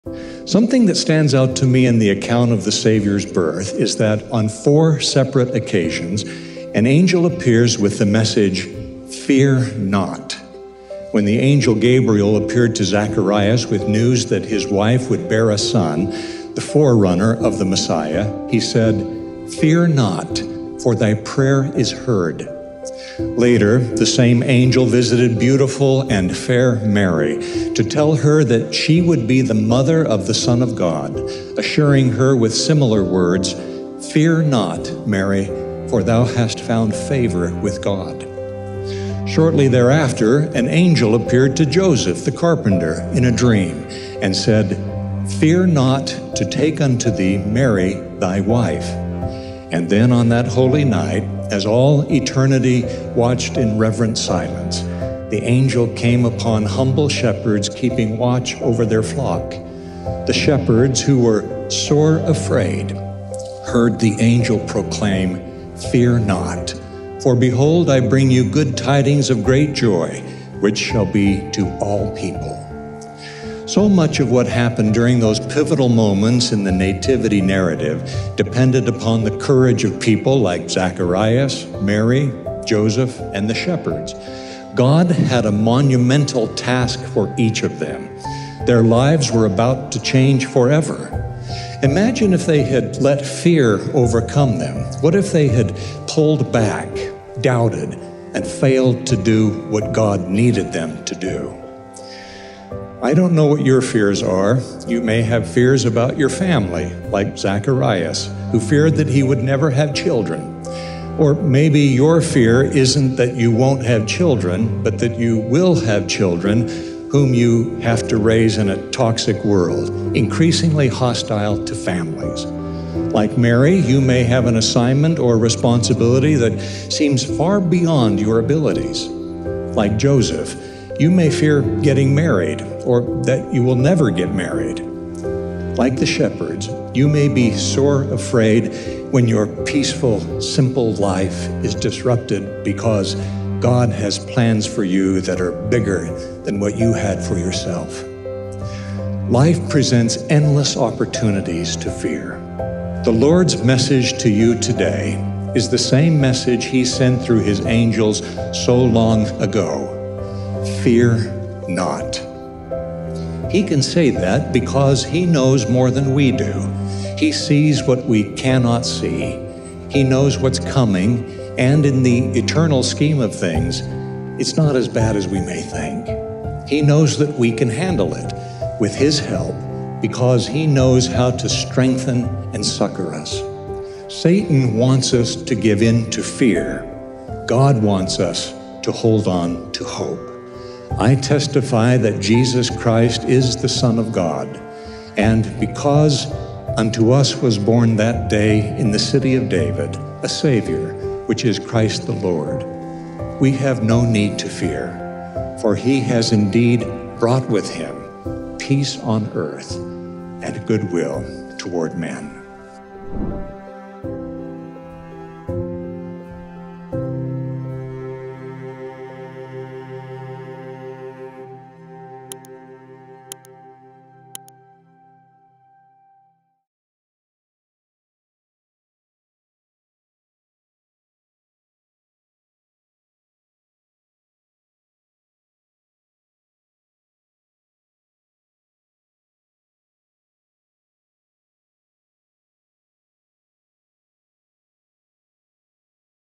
Inspiring Short: Fear Not: Good Tidings of Great Joy | Lloyd D. Newell
Click "Show more" to find the links to the speech and podcasts.

We all have fears and worries about our futures, but we can learn from the story of the Savior's birth to have courage and “fear not.”